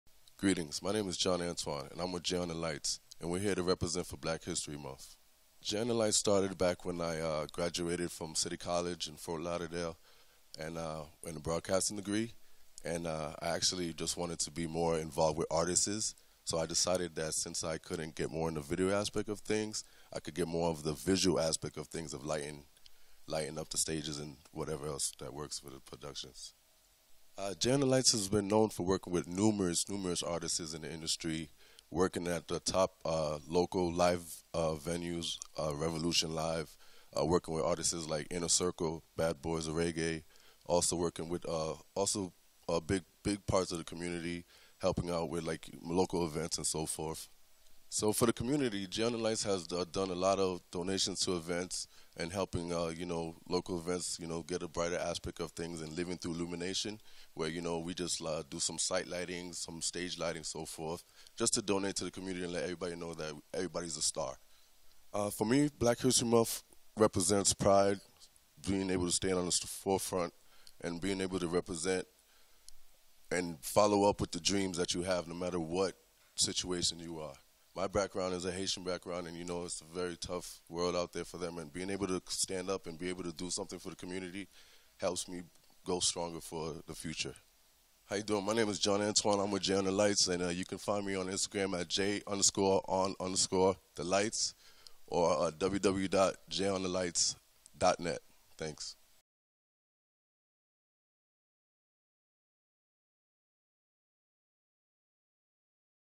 99 JAMZ Salutes Black-Owned Businesses: J ON THE LIGHTS
All throughout February, 99 JAMZ is paying homage to Black-
owned businesses across South Florida. Today's honoree is Jon Antoine of J ON THE LIGHTS. Watch and listen to his story now!